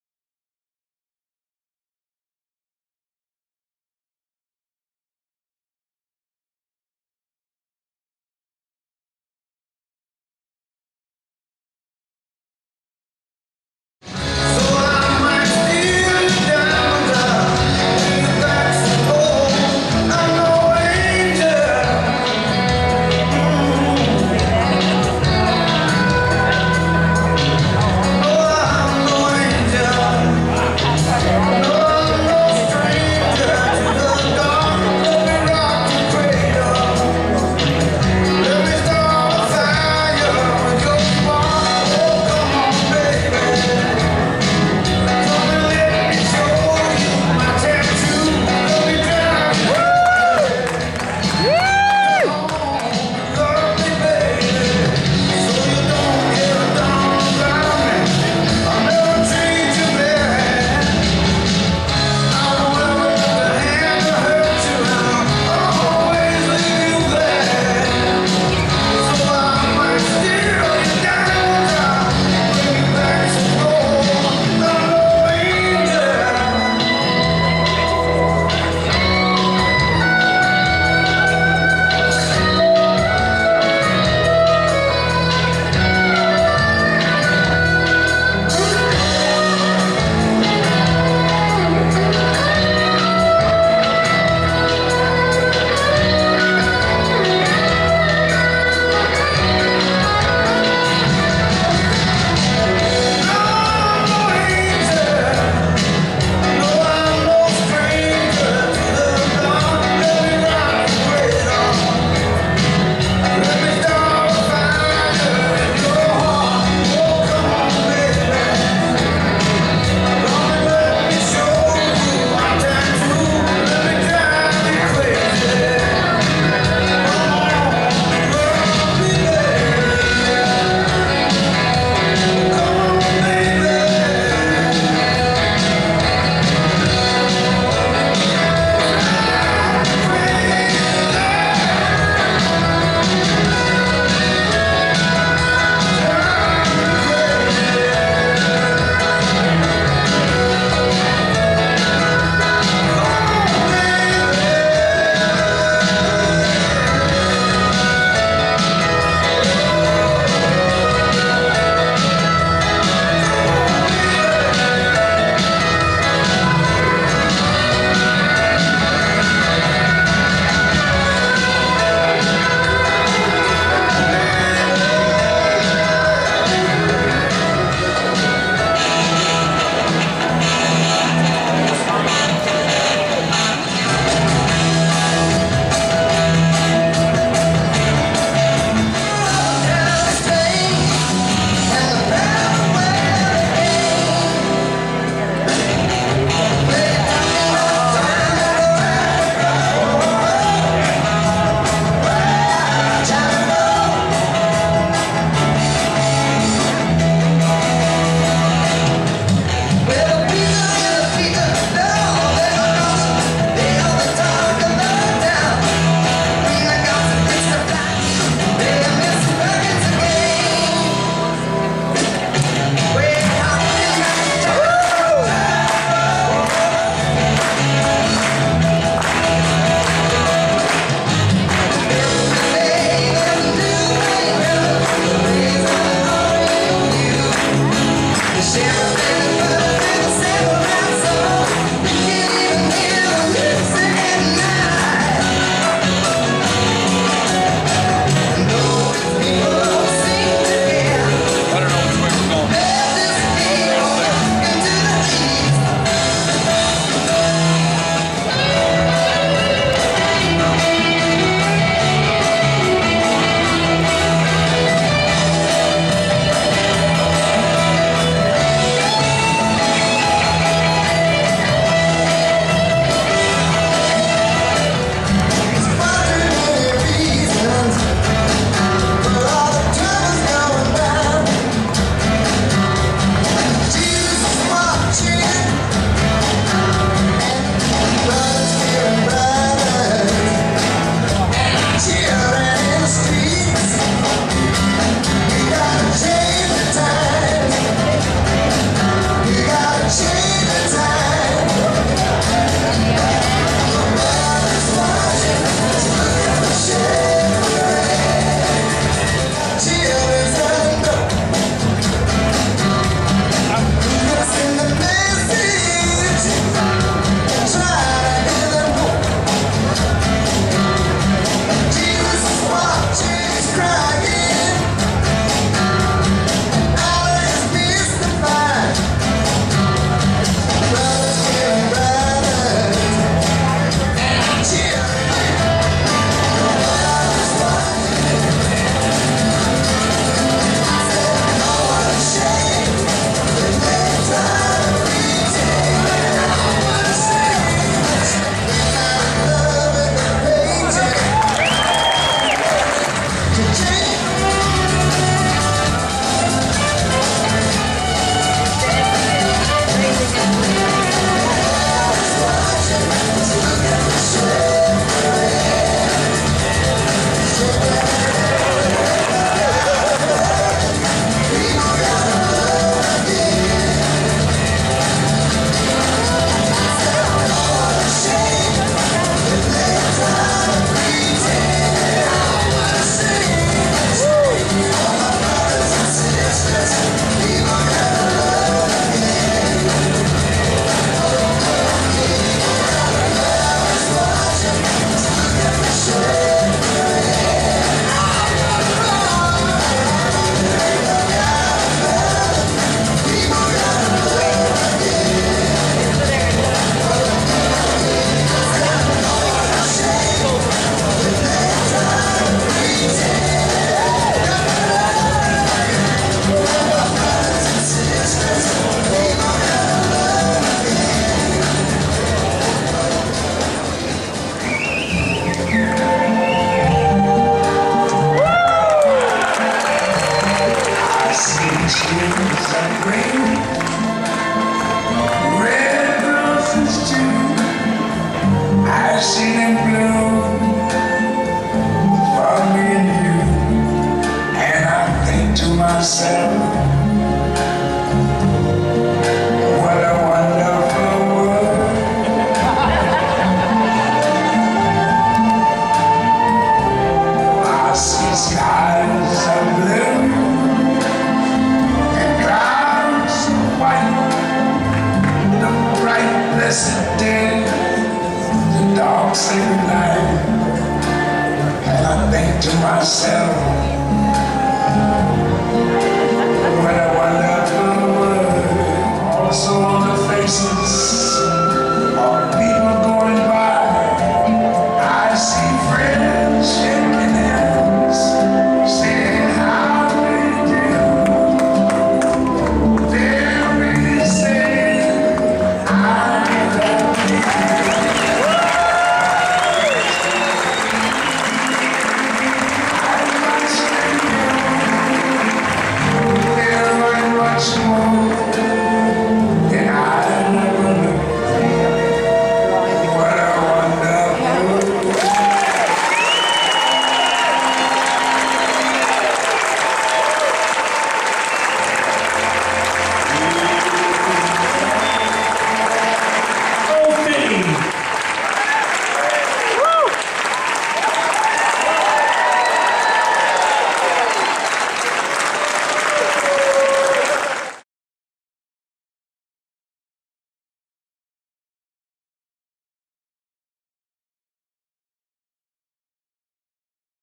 Randall Hedden Paints Michael Finney Live!
Master magician and comedian, Michael Finney is honored at the 2011 Dry Heat Classic with a live, action painting by Randall Hedden of Tributeart Live!